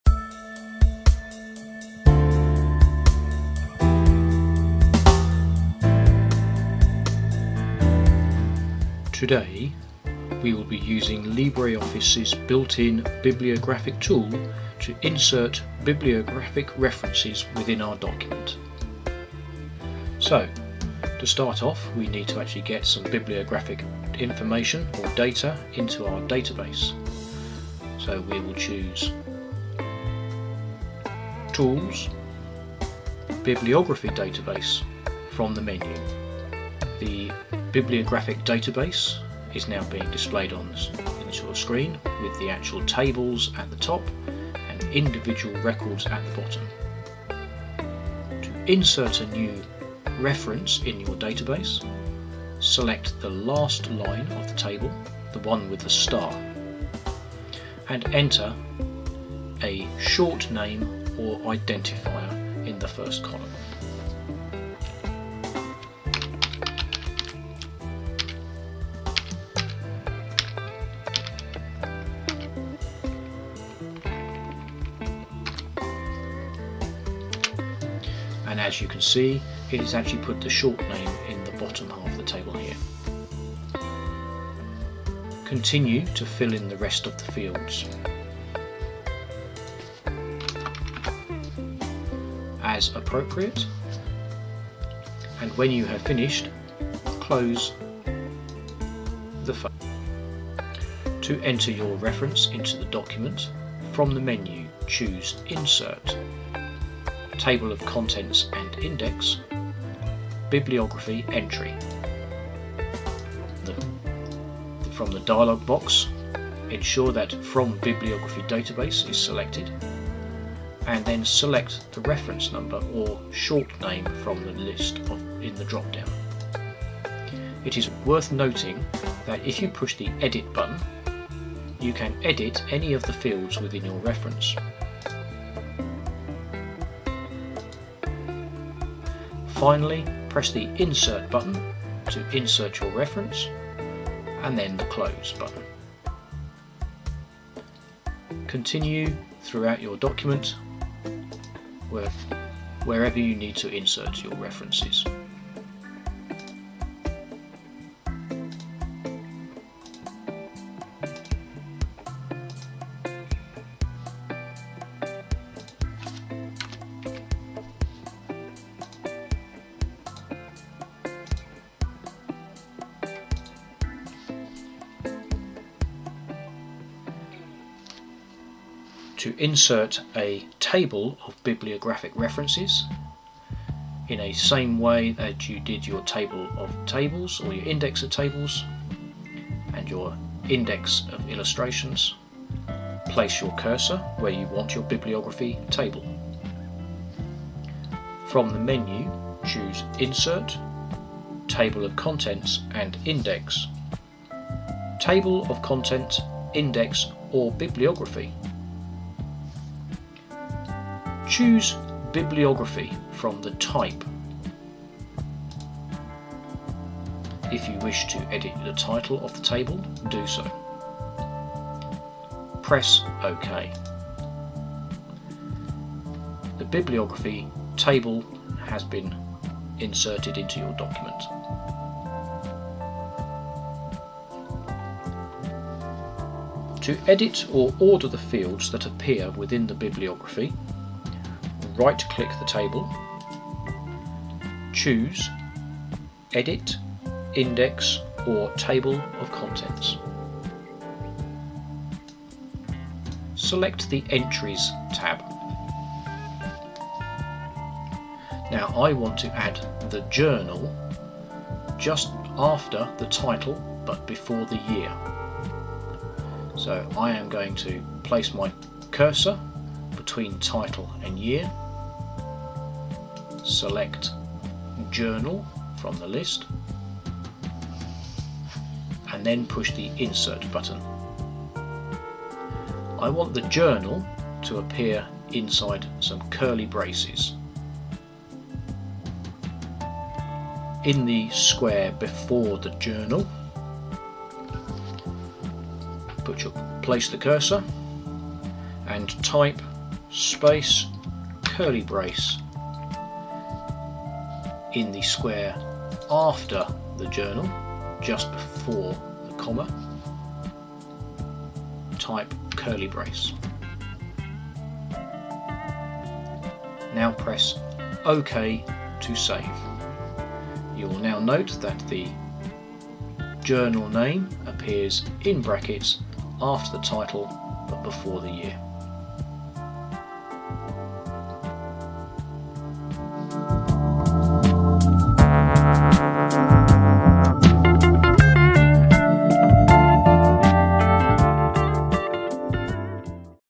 LibreOffice Tutorial 05 Inserting Bibliographic References
In this video we use LibreOffice's built in bibliographic database to add bibliographic references to our document and insert a Bibliography Table at the end.